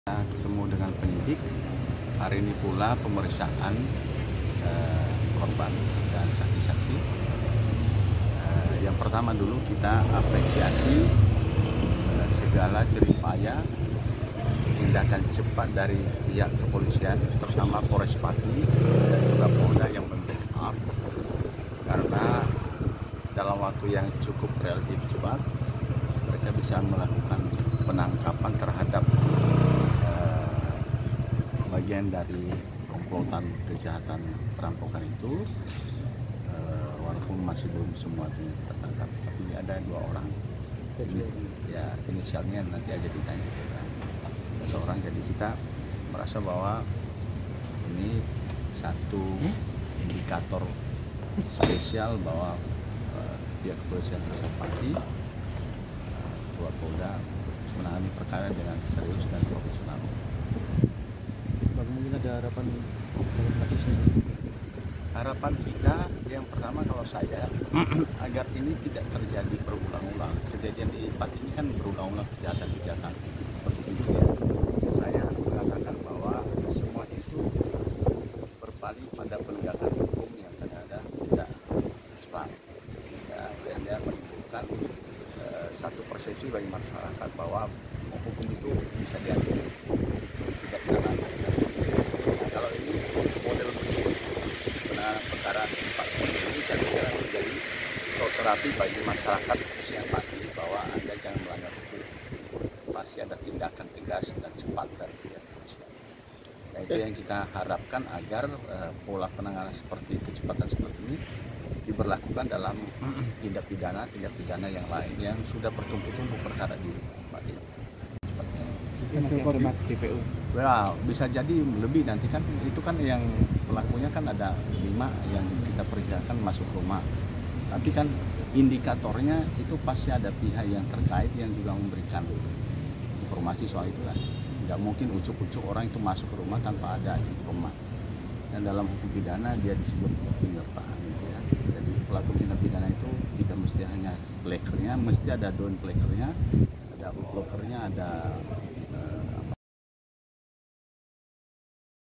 Kabar Kriminal | 2 P3ncur1 Emas 1 Kg di Pucakwangi Pati Ditangkap Polisi | Sisanya Buron
Wawancara dengan pengacara korban Nimerodi Gulo terkait perkembangan penangkapan perampokan emas di Desa Sukopuluhan Kecamatan Pucakwangi Pati pada Senin (3/6) lalu.